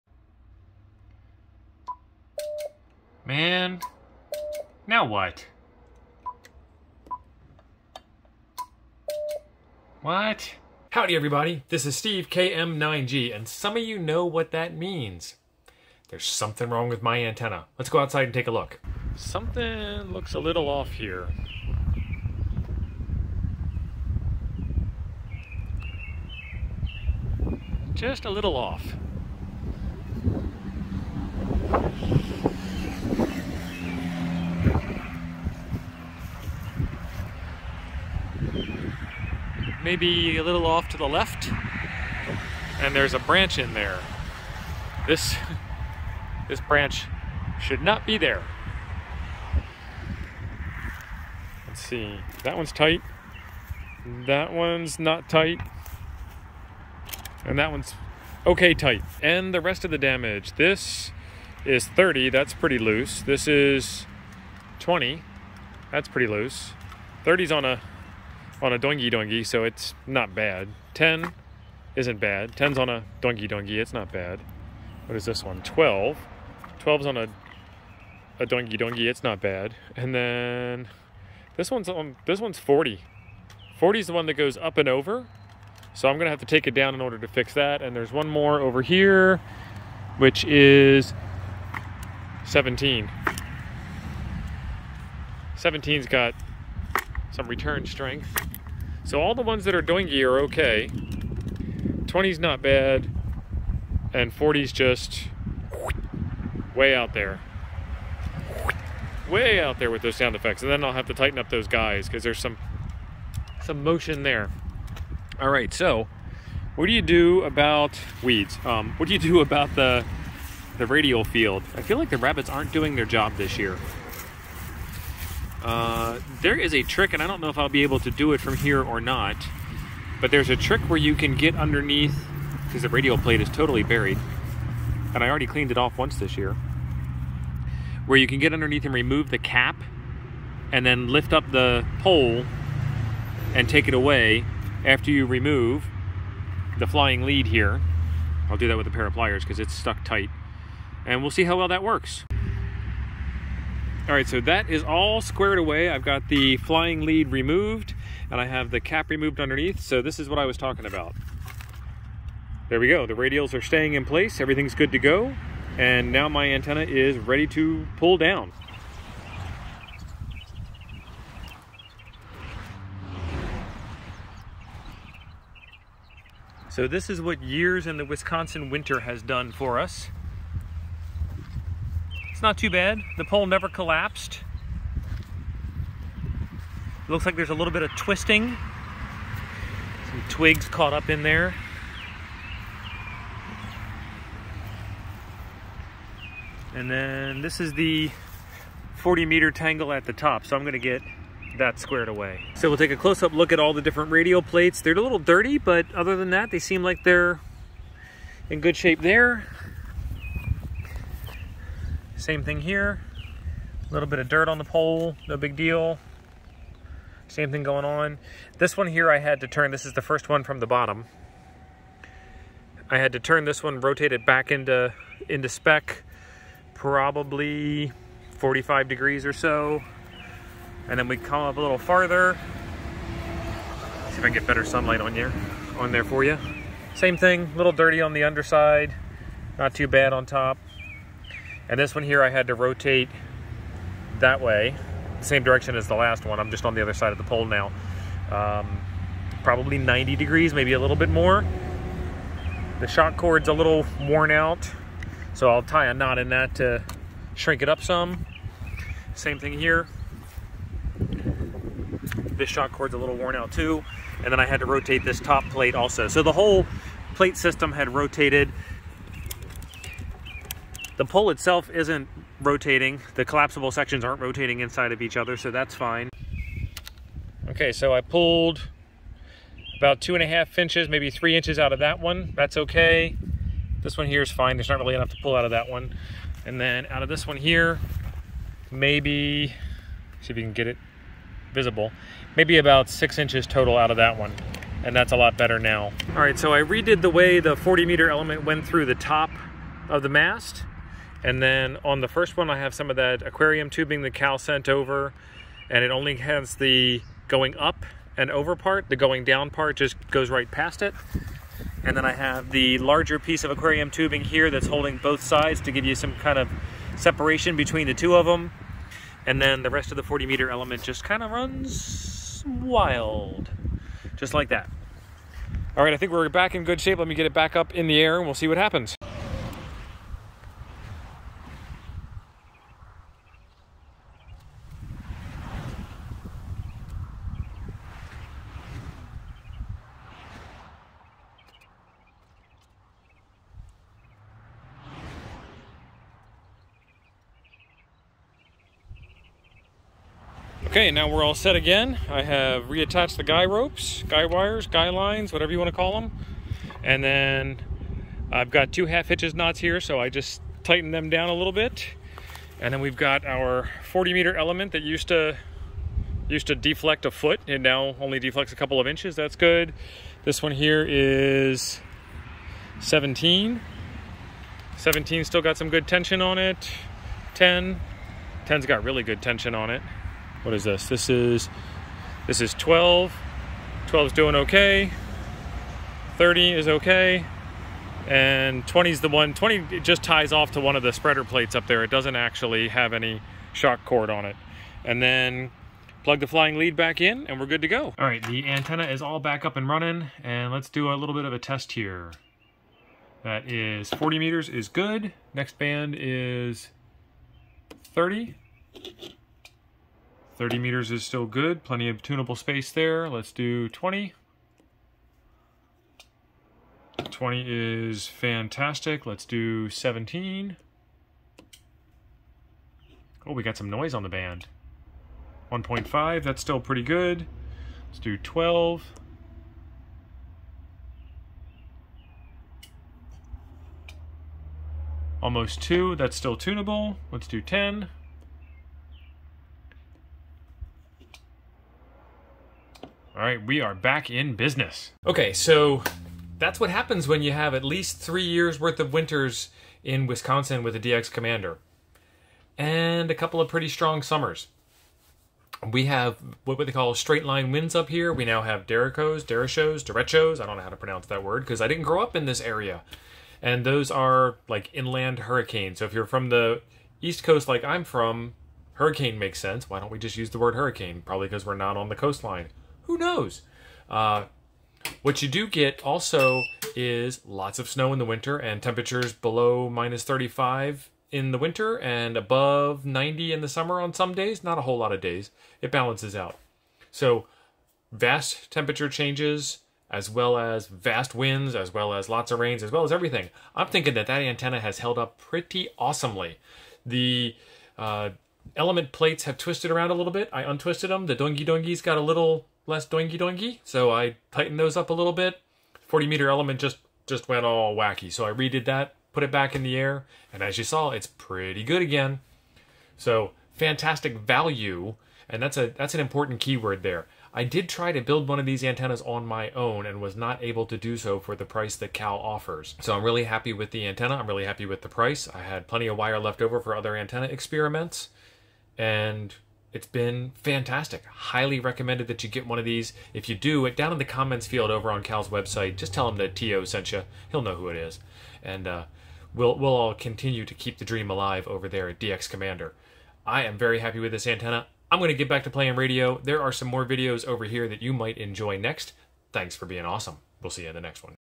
The Antenna Gods Did Not Spare Me
After at least 3 years, this is what a DX Commander looks like.  I have to take it down, inspect and make some repairs.  How did it fair?